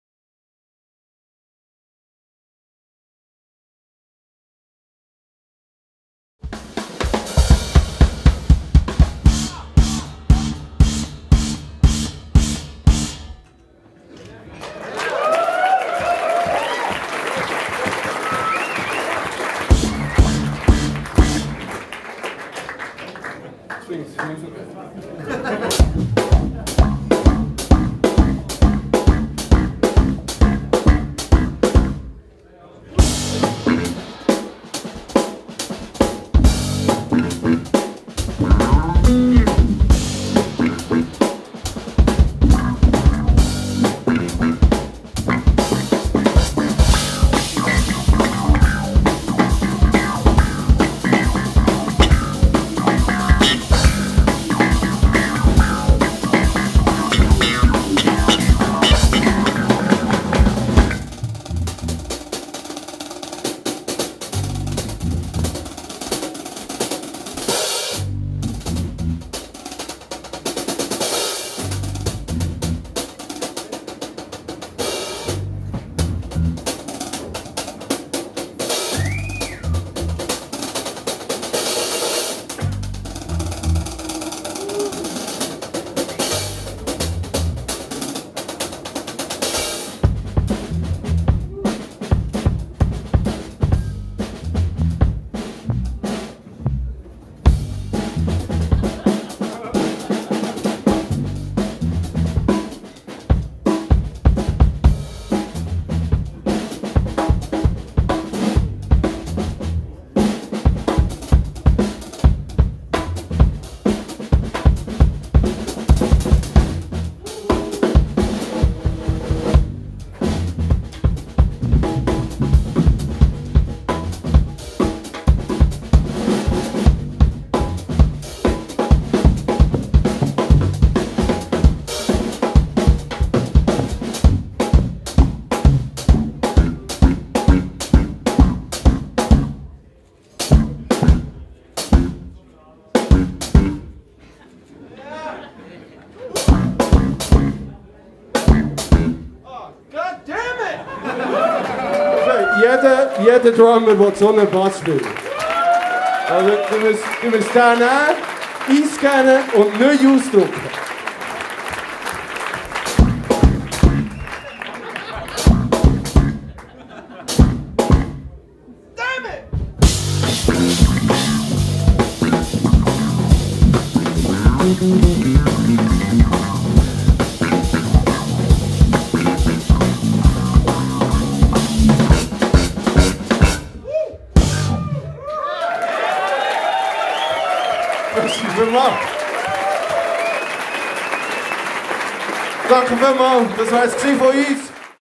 J.J. Flueck & P. Kaeser @ Drum Festival Switzerland - Official Trailer
J.J. Flueck & P. Kaeser (aka GROOVE TWINS)
Live @ Drum Festival Switzerland 2014
Official Trailer

J.J. plays Paiste Cymbals, Lauper Drums, Vic Firth Drum Sticks, Remo Drumheads and Roland Electronics
P. plays Eden Amps, Fender & DuVoisin Basses